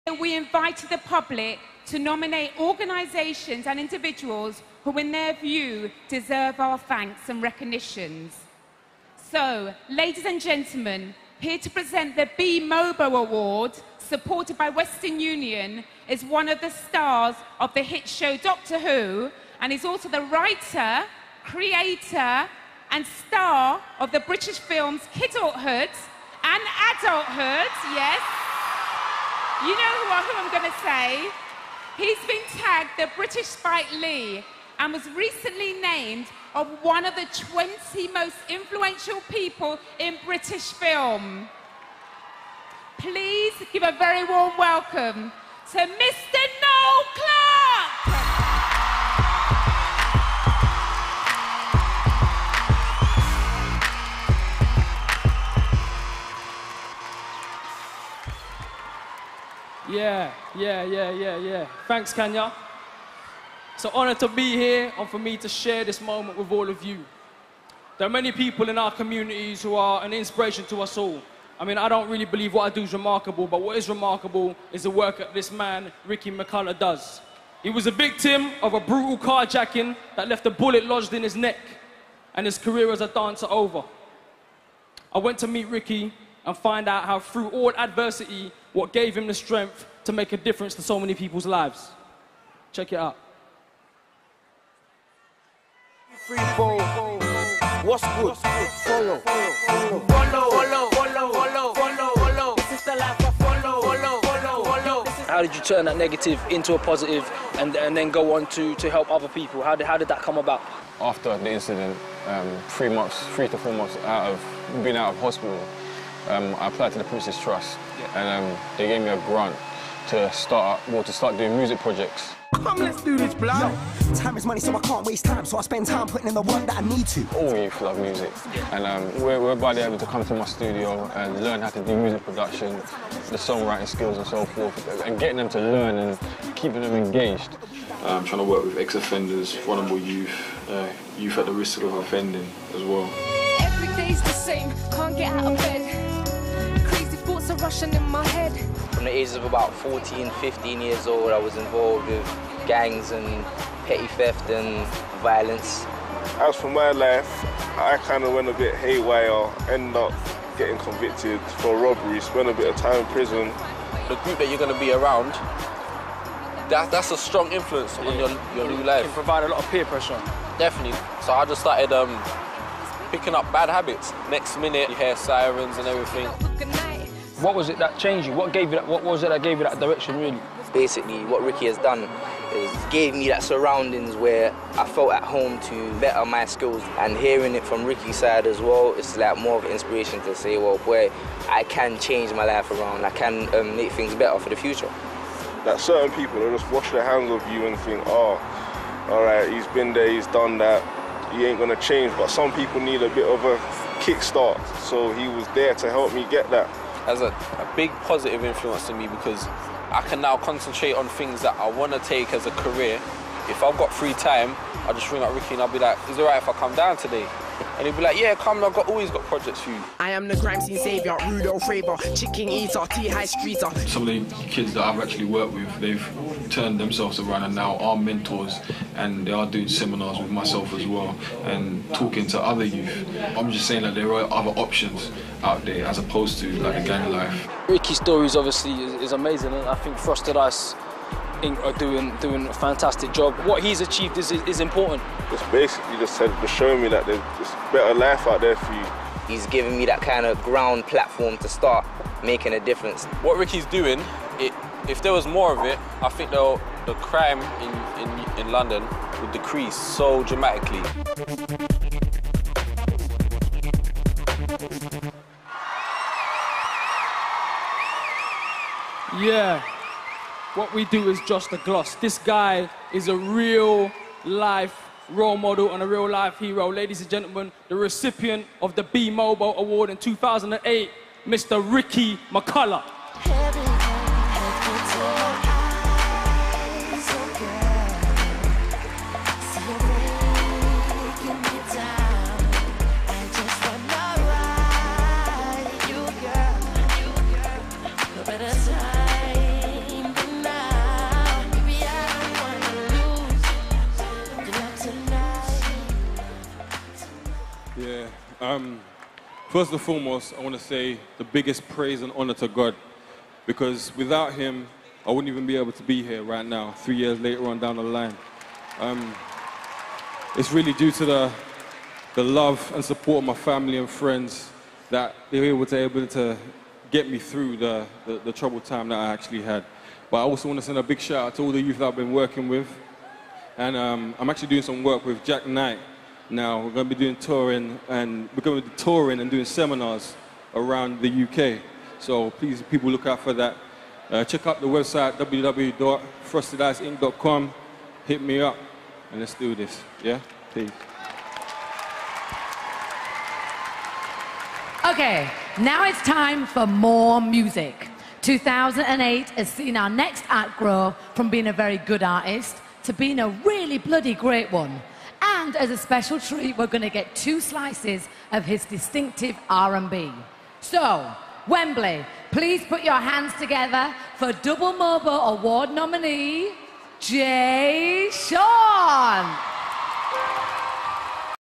Mobo 2008 BEMOBO Award
Mobo 2008 BEMOBO Award Rick 'Frost' McCalla collecting his Award